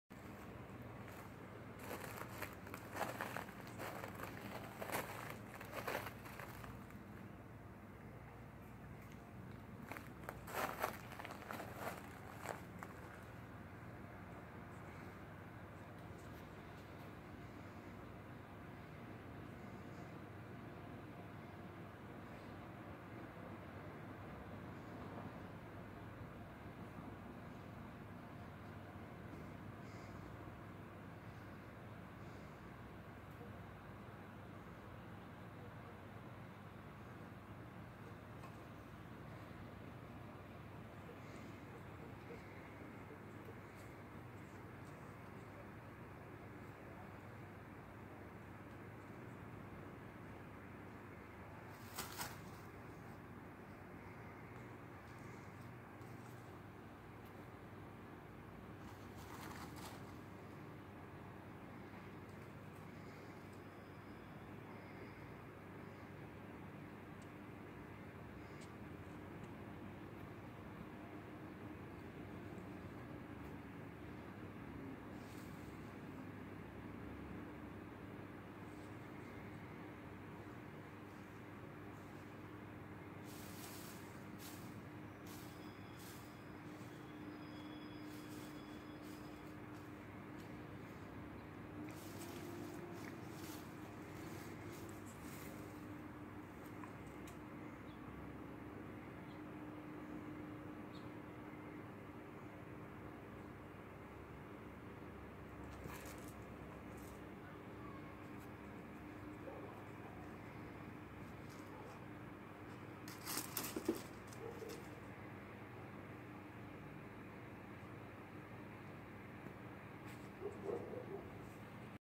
Rollie and Princess Cat Playing Outside
Rollie and Princess cats playing in Arizona. Hiding in plants, climbing on rocks and chasing each other. They are settling into Kingman, Arizona nicely.